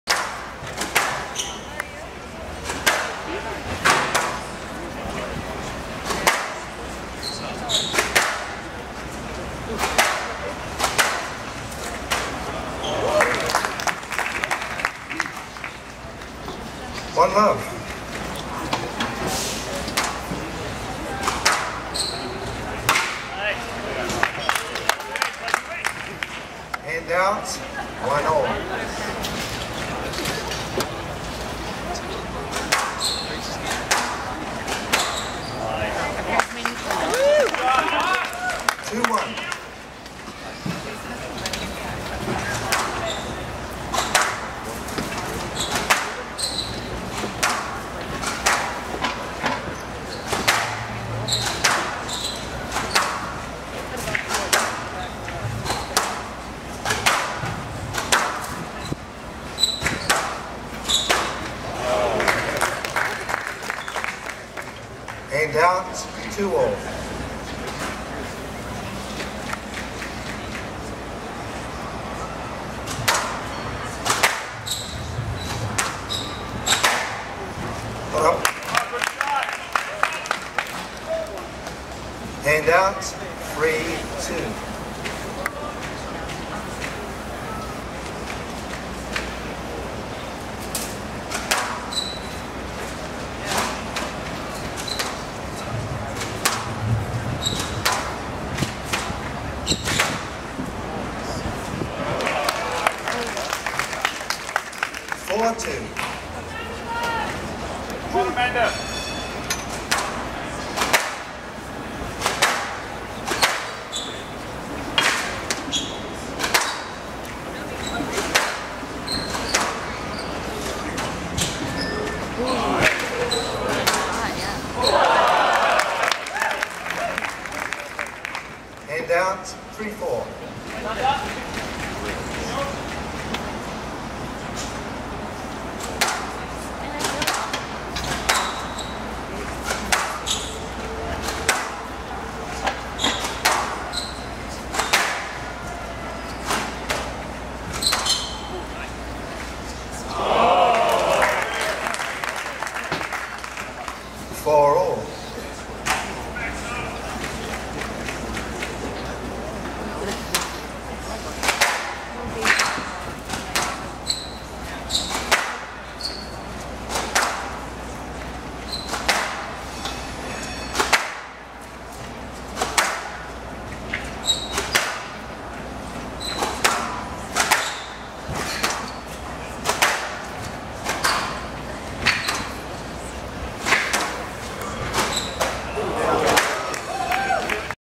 2-Semis-Amanda Sobhy [USA] d. Nicol David [MAS] squash
2-Semis-Amanda Sobhy [USA] d. Nicol David [MAS] squash

Netsuite Open WSA
San Francisco
Justin Herman Plaza
Sobhy d David 10-12, 11-5, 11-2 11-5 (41m)

MVI 4547